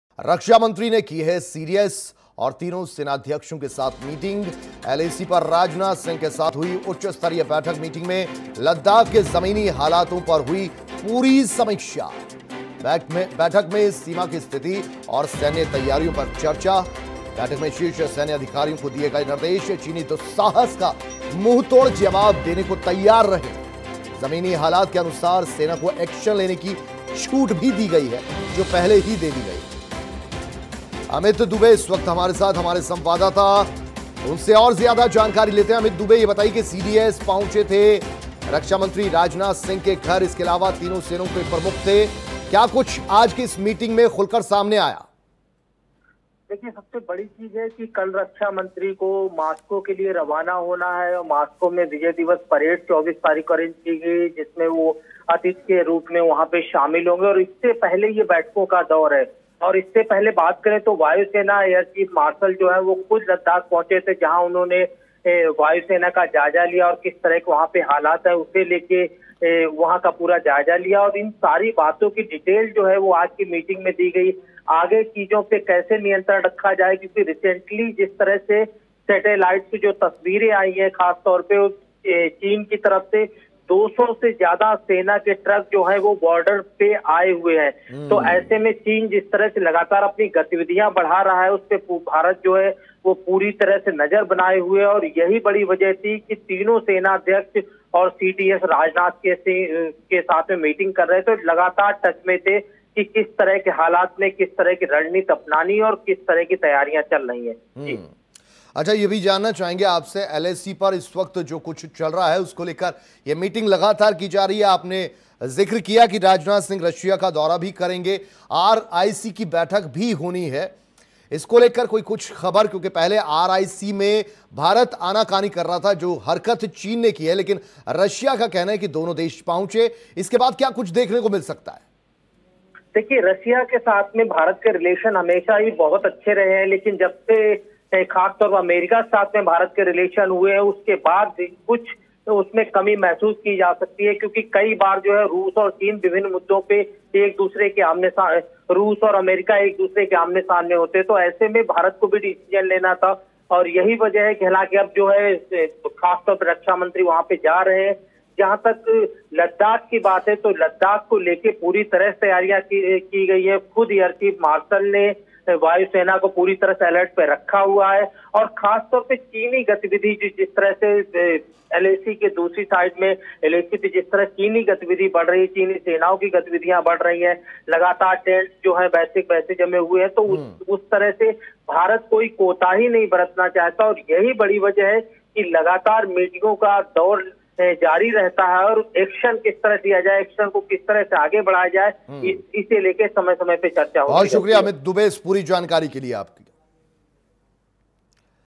LAC पर सेना को एक्शन लेने की छूट - राजनाथ सिंह ने की सेनाध्यक्षों के साथ मीटिंग
रक्षा मंत्री ने की CDS और तीनों सेनाध्यक्षों के साथ मीटिंग
LAC पर राजनाथ सिंह के साथ हुई उच्च स्तरीय बैठक
मीटिंग में लद्दाख के जमीनी हालात की हुई पूरी समीक्षा
बैठक में सीमा की स्थिति और सैन्य तैयारियों पर चर्चा
बैठक में शीर्ष सैन्य अधिकारियों को दिए गए निर्देश
चीनी दुस्साहस का मुंहतोड़ जवाब देने को रहें तैयार
जमीनी हालात के अनुसार सेना को एक्शन लेने की छूट